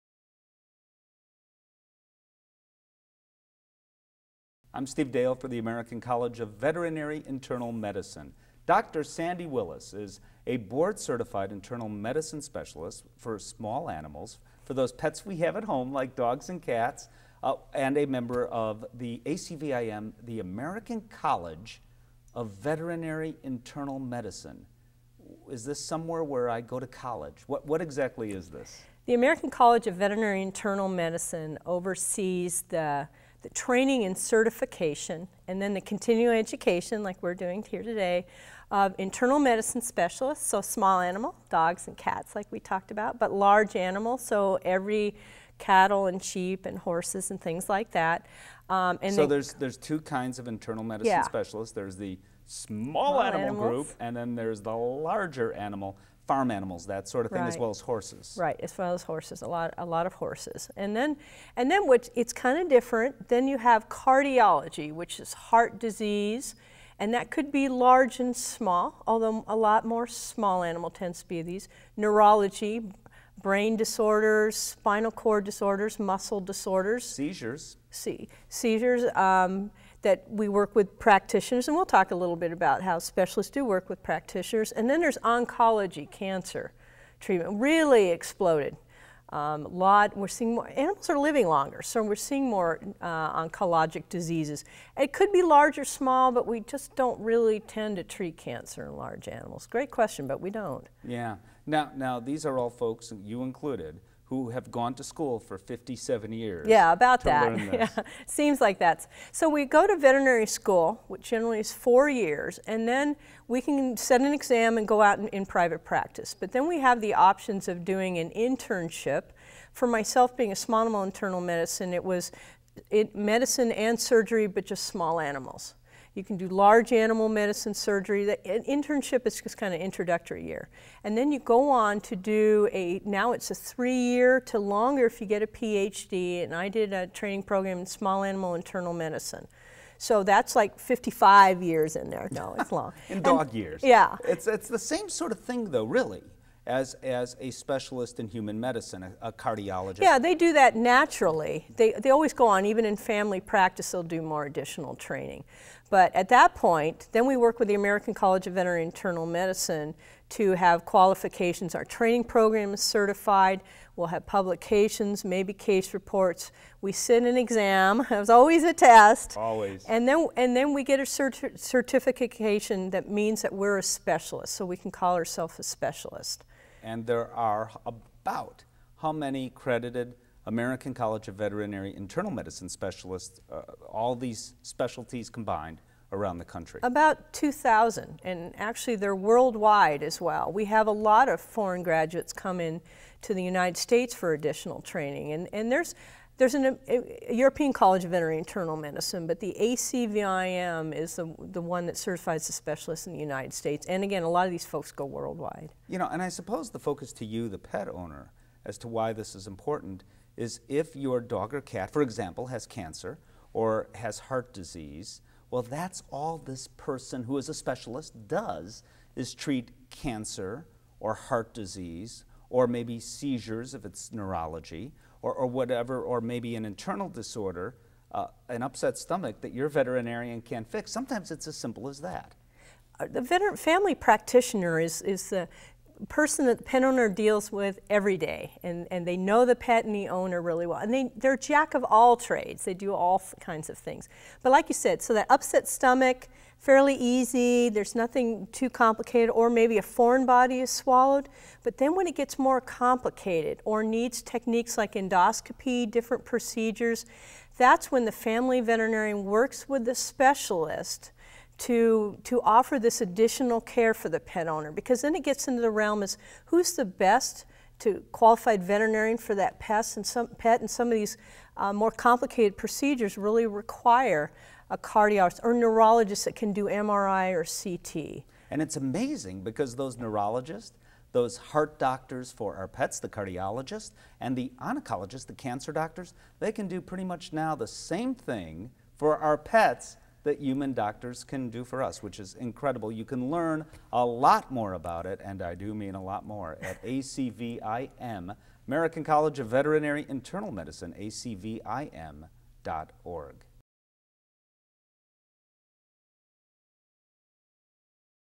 What is the American College of Veterinary Internal Medicine (ACVIM)?
The American College of Veterinary Internal Medicine (ACVIM) is a non-profit organization dedicated to improving the lives of animals and people through education, training and certification of specialists in veterinary internal medicine, discovery and dissemination of new medical knowledge, and increasing public awareness of advances in veterinary medical care.

The ACVIM is the international certifying organization for veterinary specialists in Large Animal Internal Medicine, Small Animal Internal Medicine, Cardiology, Oncology, and Neurology.